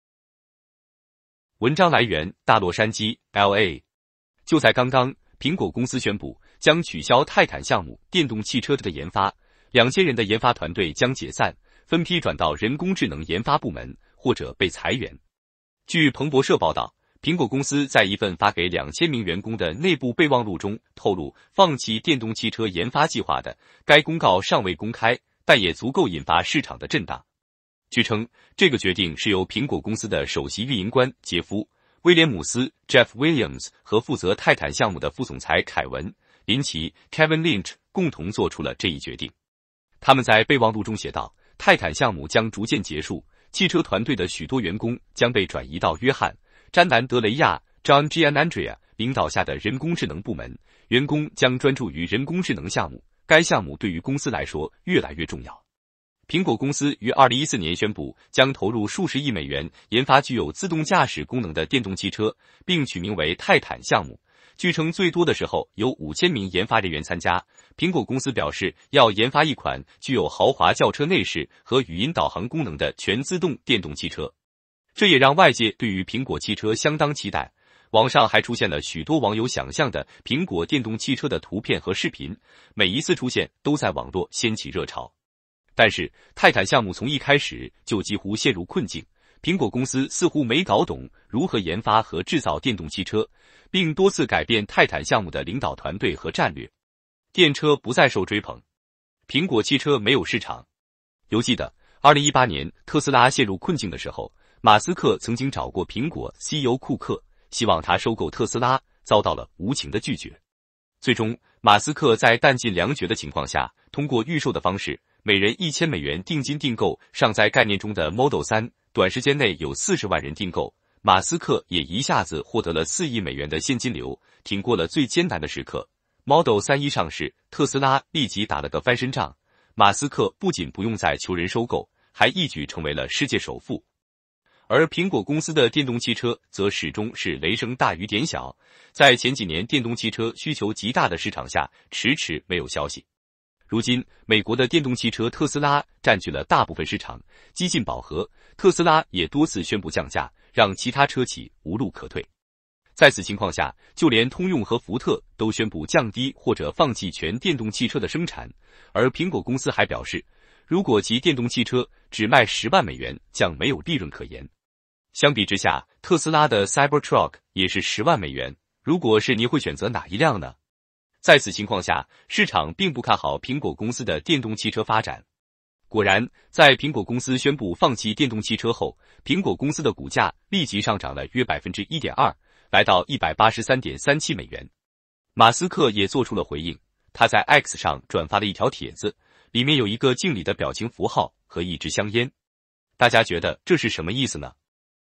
重磅！苹果宣布放弃 电动汽车项目 2000人团队解散
文章来源: 大洛杉矶LA
就在刚刚，苹果公司宣布，将取消“泰坦”项目（电动汽车）的研发，2000人的研发团队将解散，分批转到人工智能研发部门，或者被裁员。据彭博社报道，苹果公司在一份发给2000名员工的内部备忘录中透露放弃电动汽车研发计划的，该公告尚